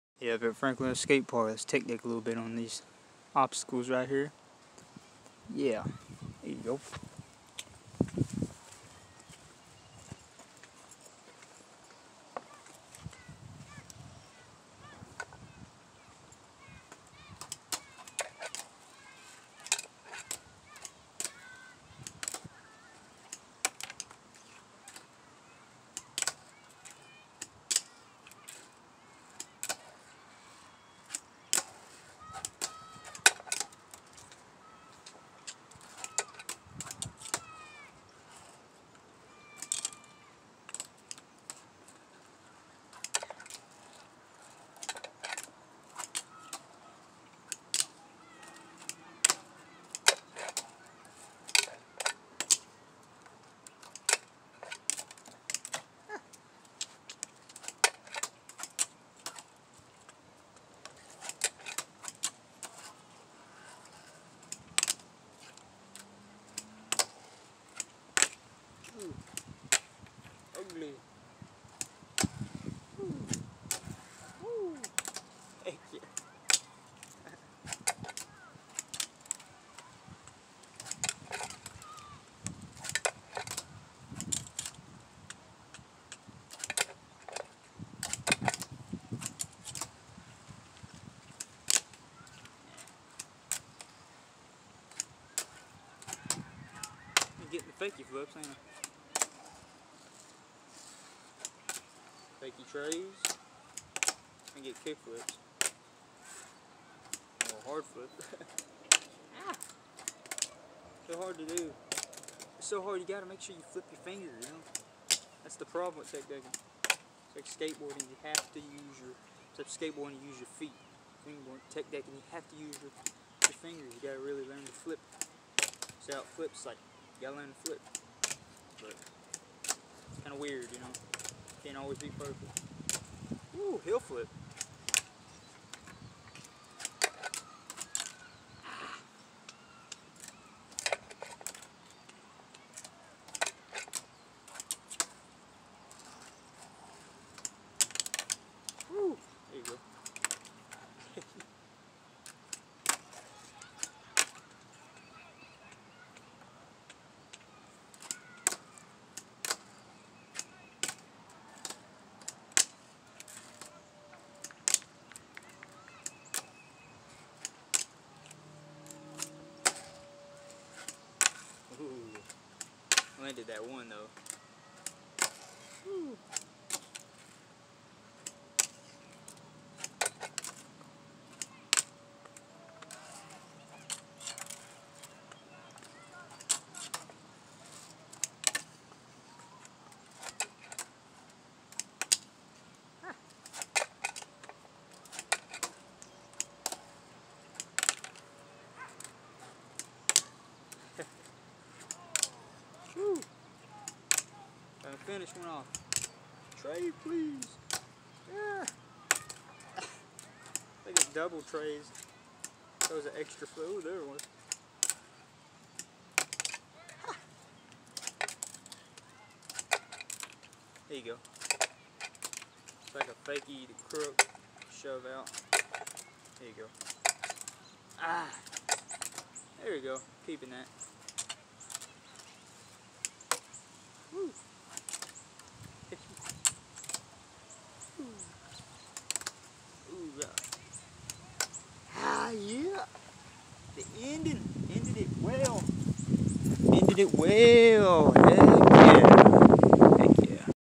At franklorino skate park on thu grey table tech decking. And I ended it well!
I'm having fun tech decking in Morristown Tennessee at franklorino skate park.